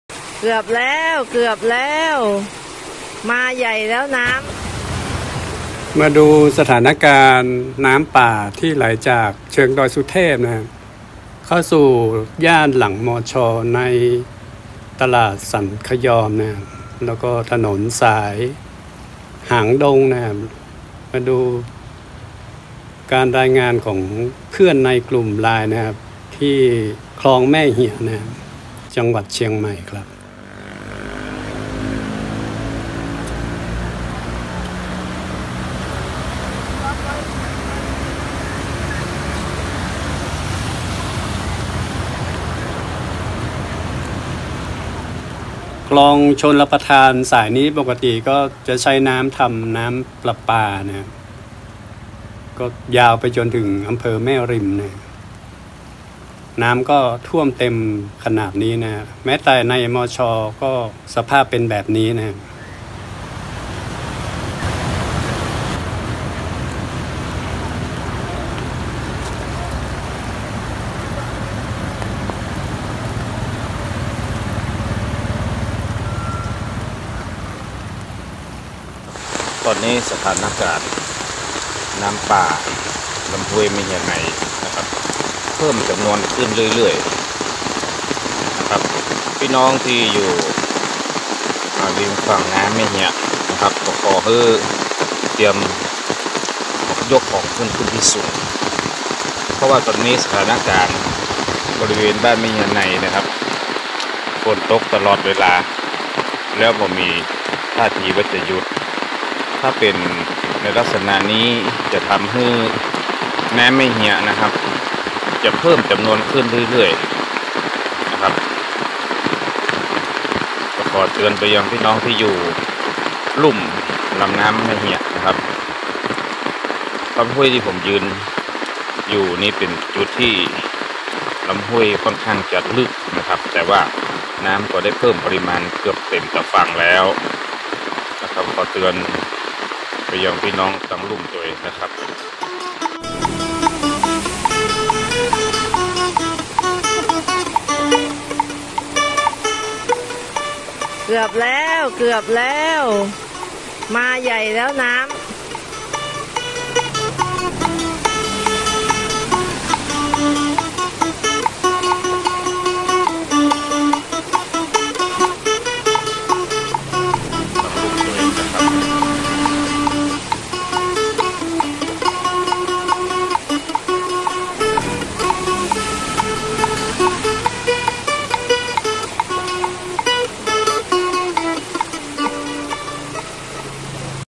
สถานการณ์น้ำป่าดอยสุเทพไหลท่วมม.ช,ตลาดสันคะยอม, หางดง
น้ำป่าจากดอยสุเทพไหลท่วมหลายพื้นที่ในเชียงใหม่